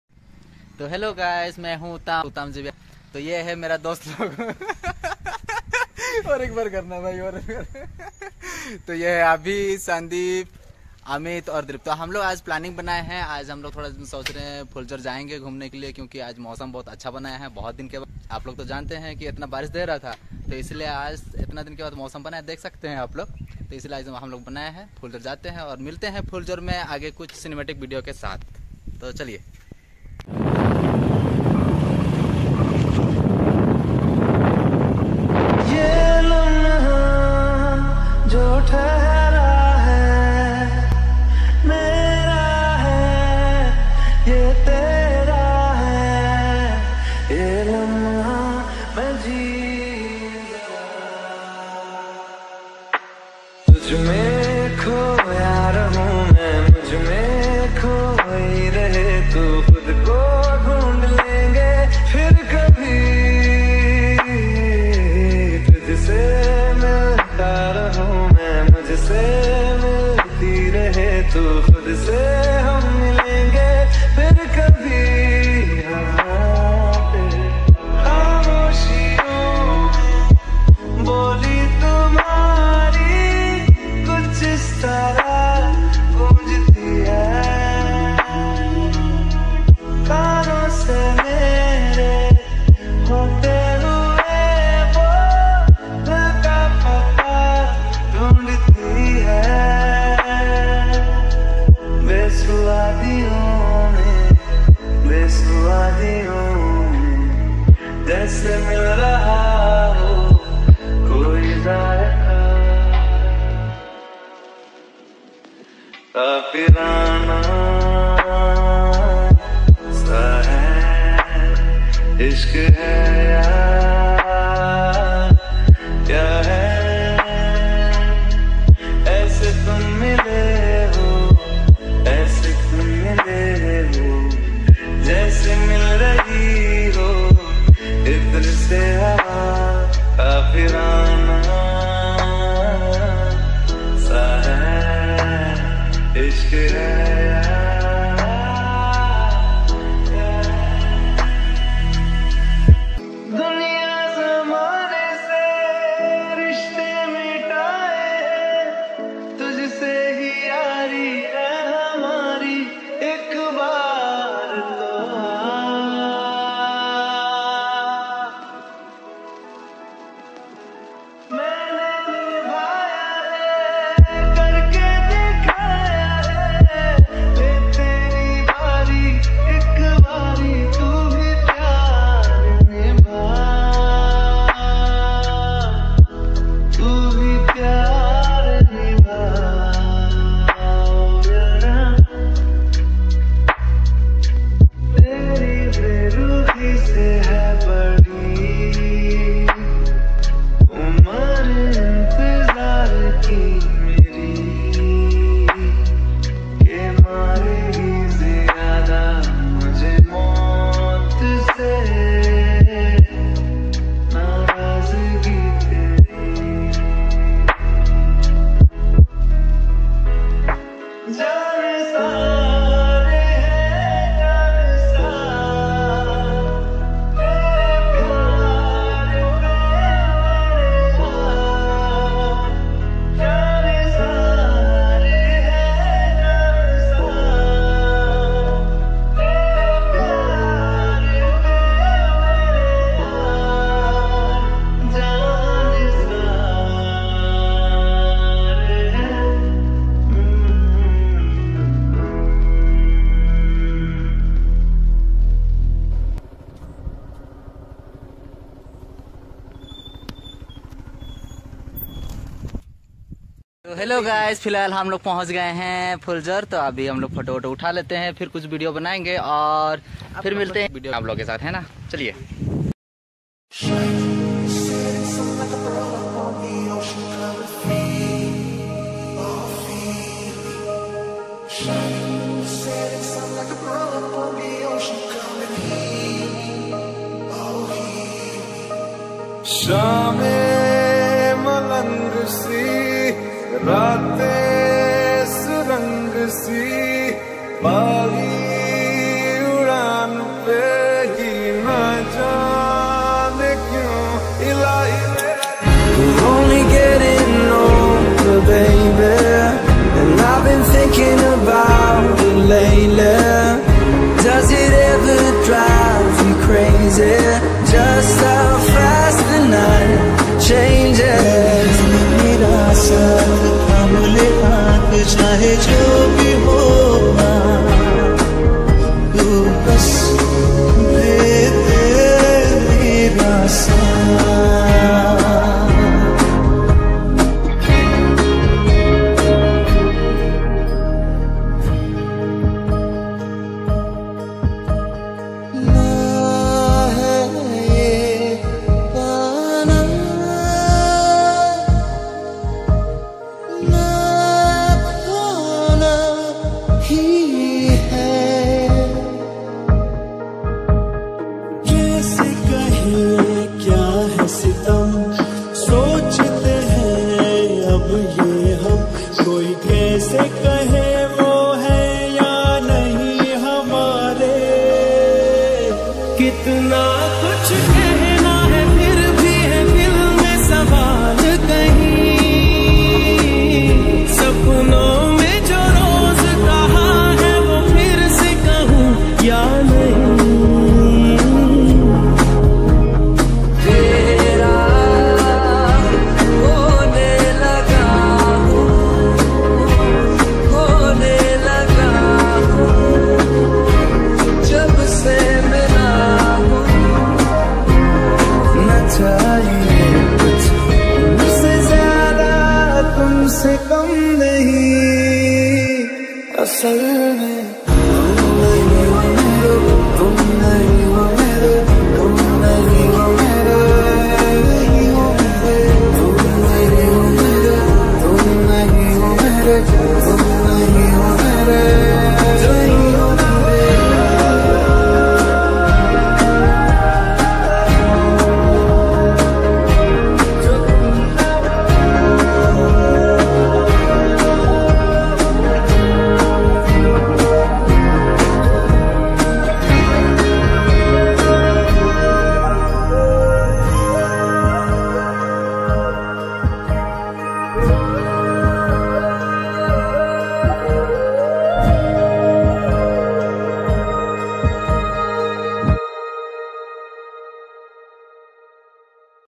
BIke Ride With Friend's From Tensa To Tantra Fuljhar// Tensa Cinematic Video// Most Cinematic video
BIke Ride With Friend's From Tensa To Tantra Fuljhar// Tensa fuljhar cinematic video// Tensa Video// Fuljhar Video// Tantra Video// Tensa Cinematic Video
Mr.x Official..